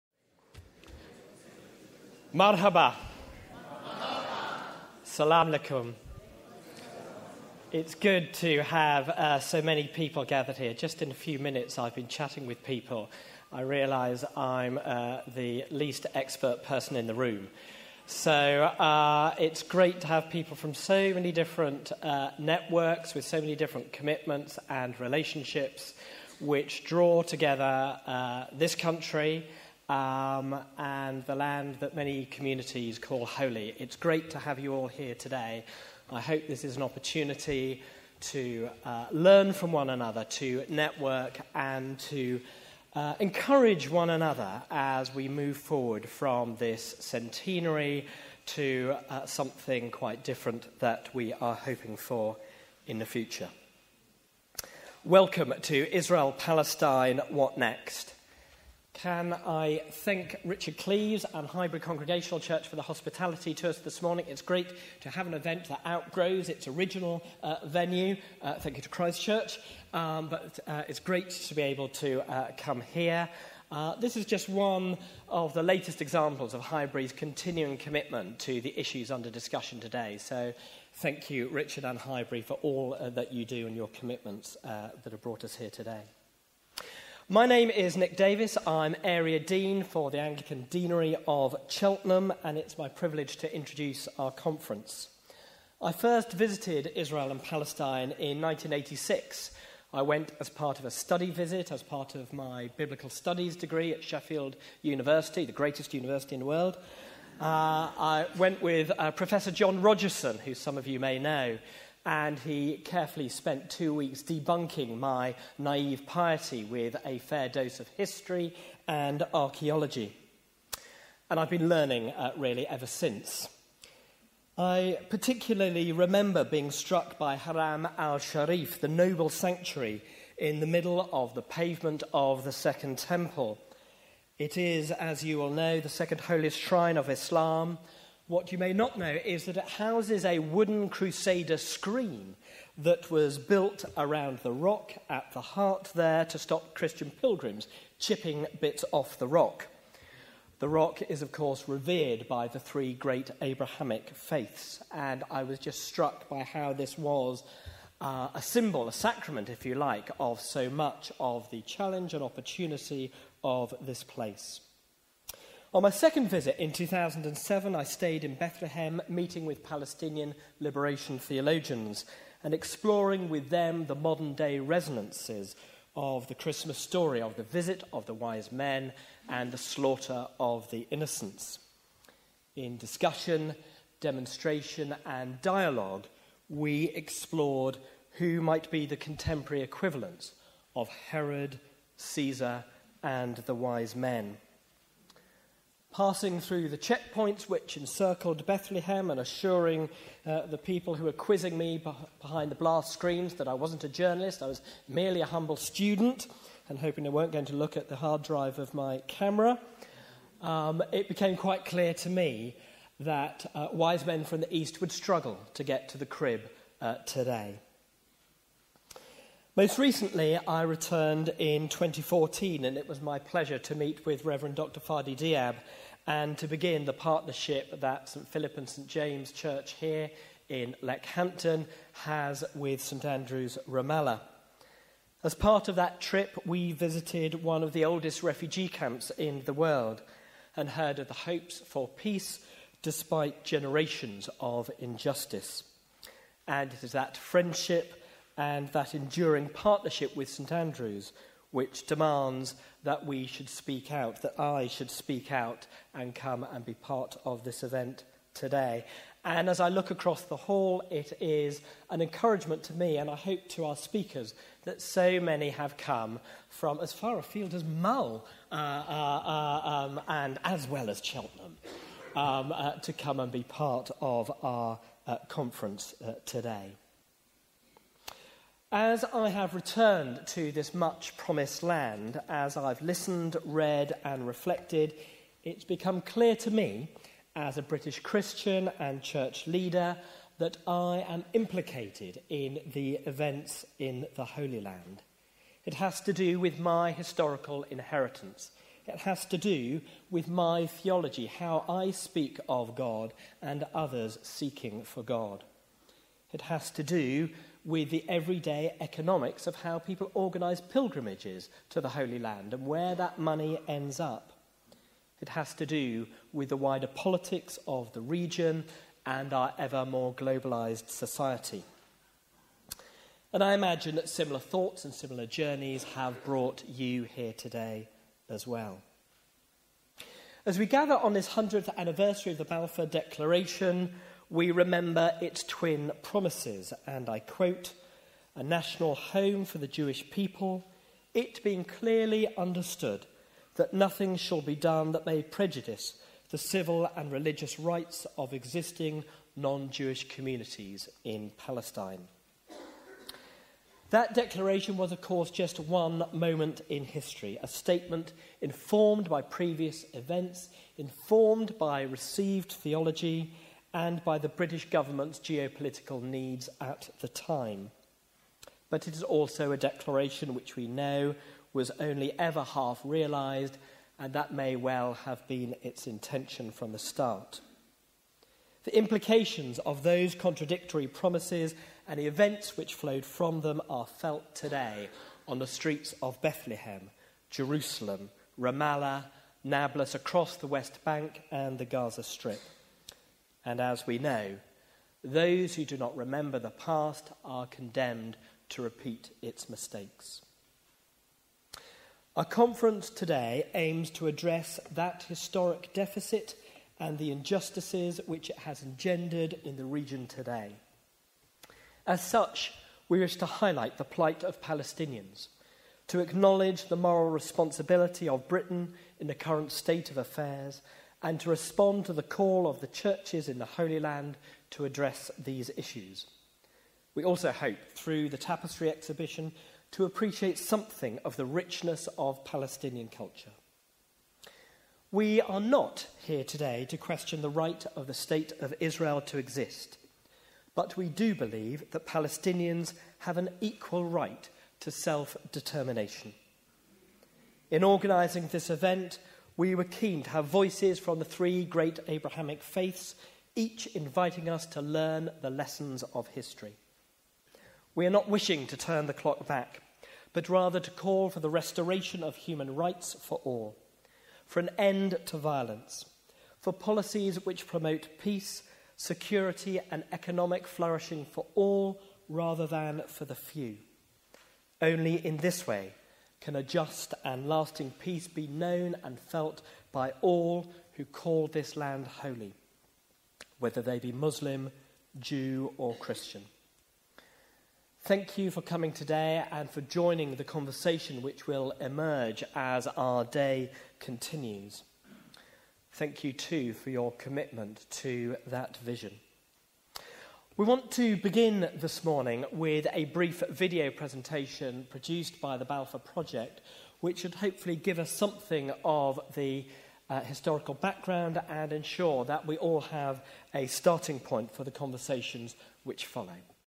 Israel Palestine - What Next?
Nick Davies's introduction to Israel Palestine - What Next? A Day Conference to mark the centenary of the Balfour Declaration